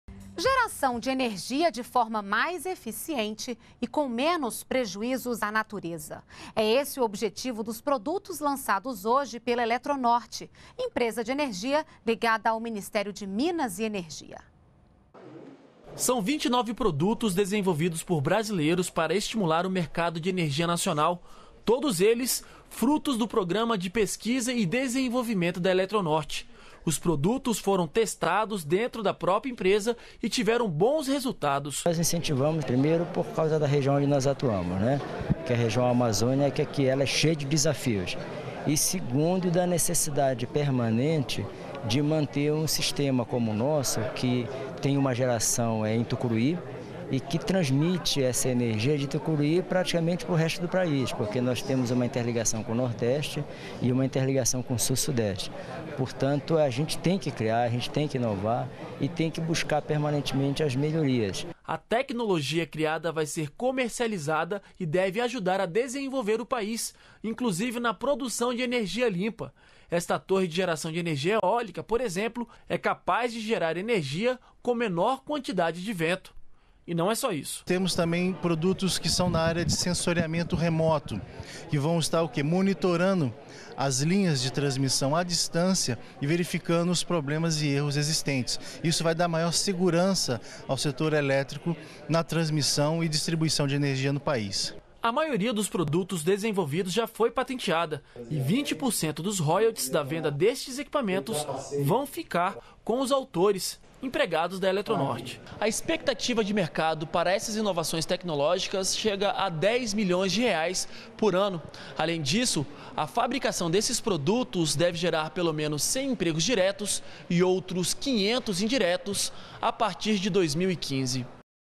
Eletronorte lança produtos que geram energia de forma mais limpa e eficiente
NOTÍCIAS - 29.01.14: Para gerar energia de forma mais eficiente e com menos prejuízos ao meio ambiente, a Eletronorte, empresa ligada ao Ministério de Minas e Energia, lançou 29 produtos. Frutos de pesquisas e testes dentro da própria empresa, as inovações serão comercializadas e contribuirão para o setor de energia limpa. Os royalties da venda das inovações desenvolvidas pelos empregados para o mercado serão distribuídos pela Eletronorte e os autores.